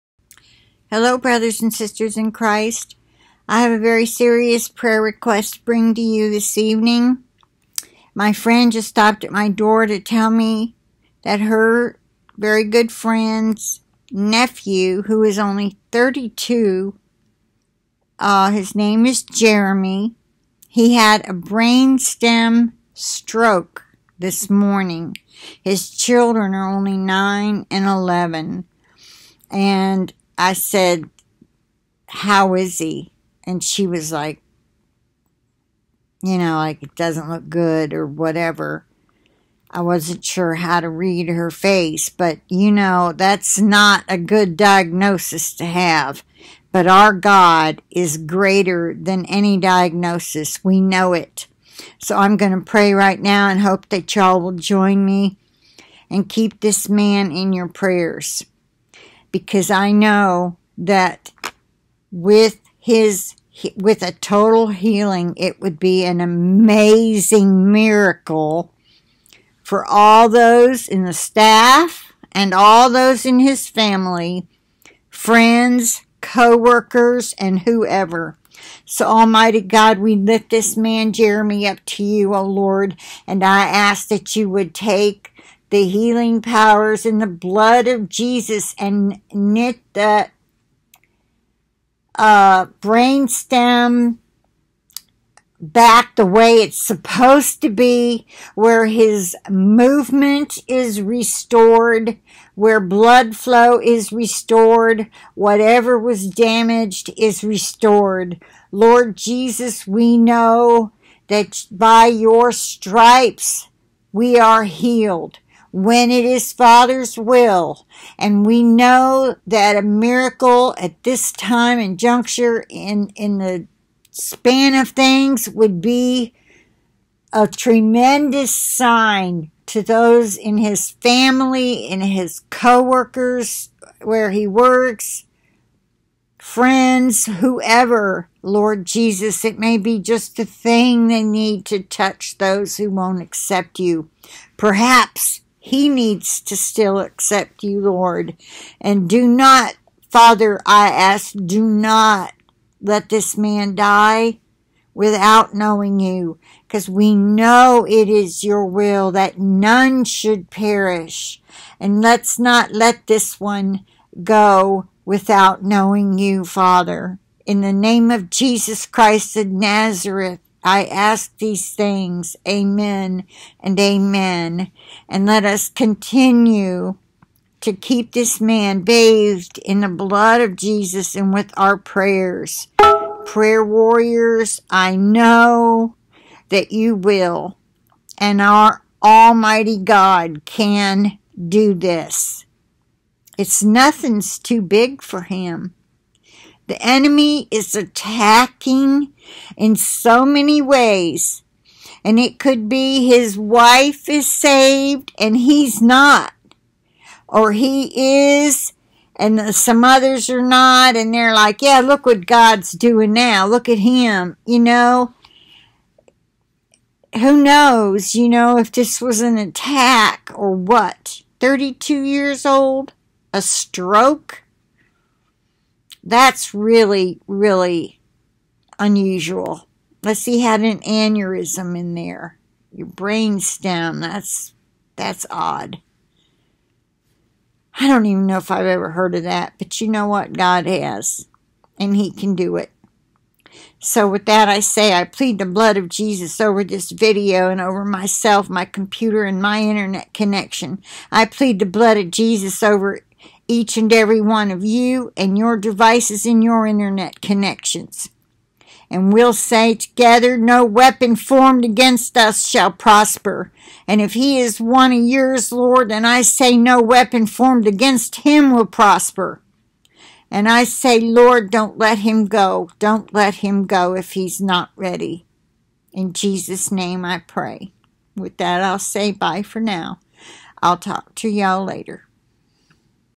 URGENT PRAYER REQUEST FOR 32 YR. OLD FATHER HAD STROKE THIS MORNING. NOT GOOD, BUT GOD IS GREATER!!
Please keep Jeremy in your prayers, brothers and sisters. This miracle would benefit so many spiritually. Almighty God, please bring this man back, as perfect as before the stroke! In Jesus Name I pray, Amen.